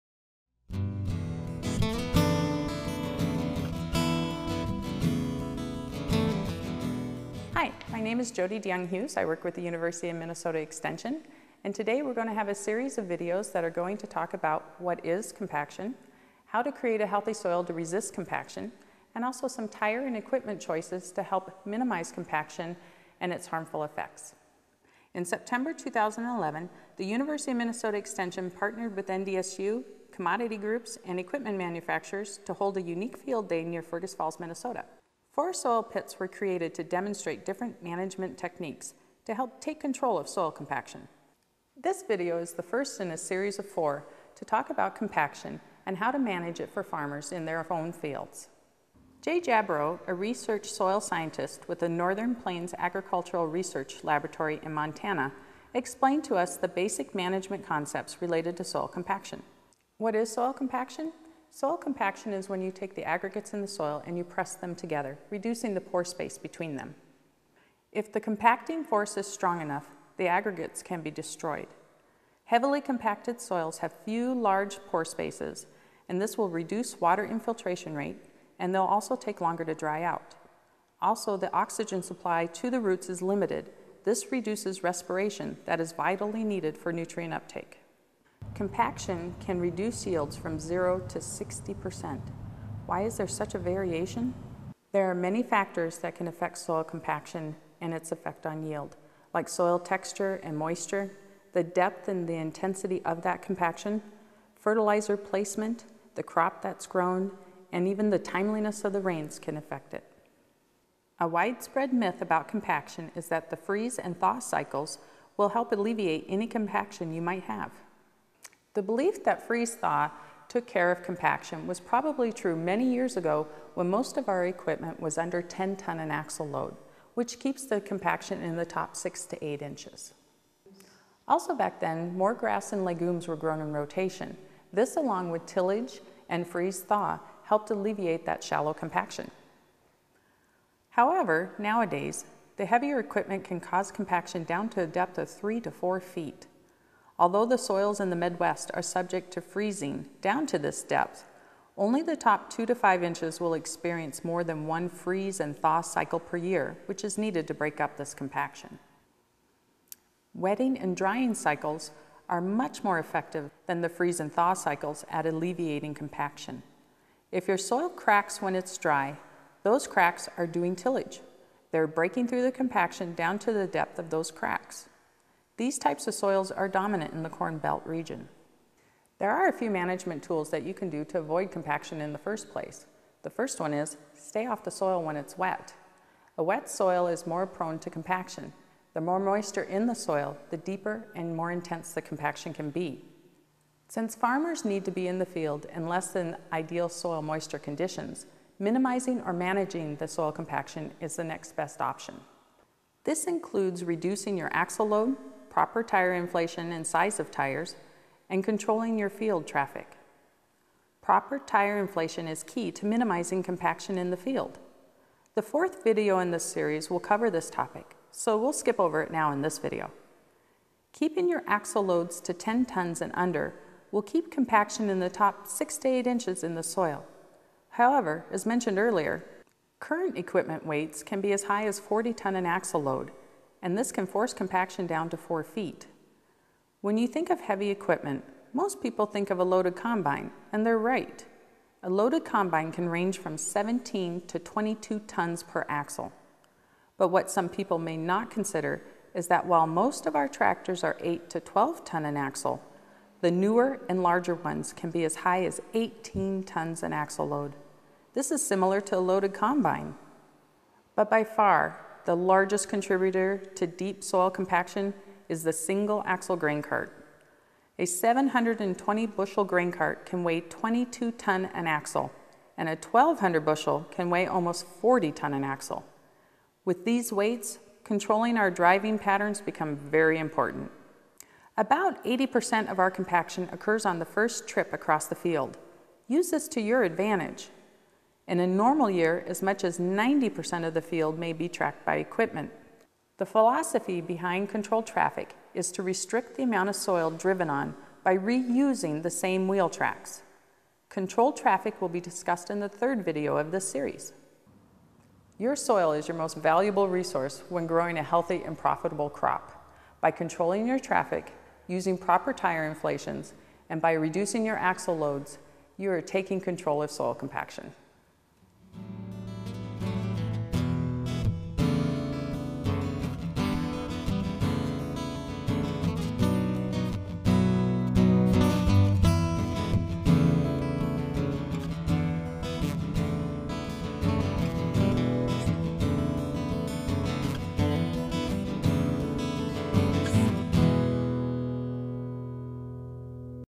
1 Soil Compaction
This video will describe soil compaction, its effects, and a few ways to avoid compaction.  The freeze-thaw cycle myth will be addressed as well as a brief introduction of the next 3 videos in the series.